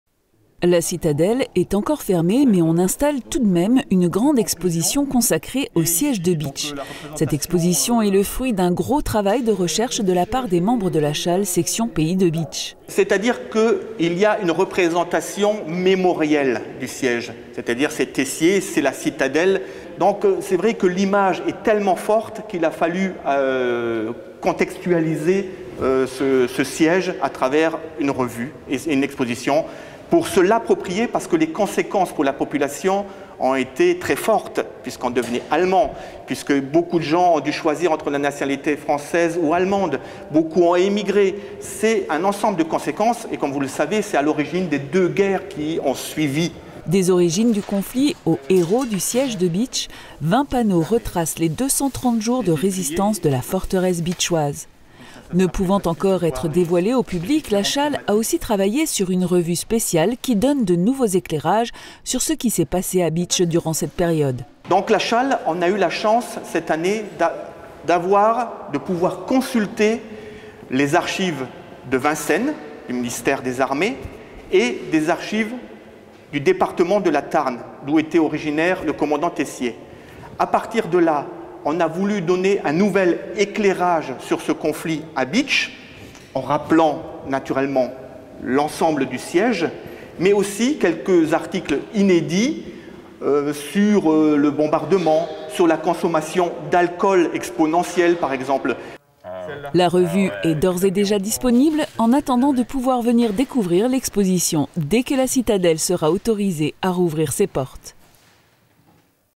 La SHAL commémore la fin du siège de Bitche avec une revue et une exposition
La ville de Bitche s’apprête à commémorer la fin du siège de Bitche. Hier, la SHAL a installé son exposition et une revue consacrée à ces 230 jours de résistance du mois d’Aout 1870 au 27 mars 1871 sort aujourd’hui.
Réalisation : Catherine HENRICH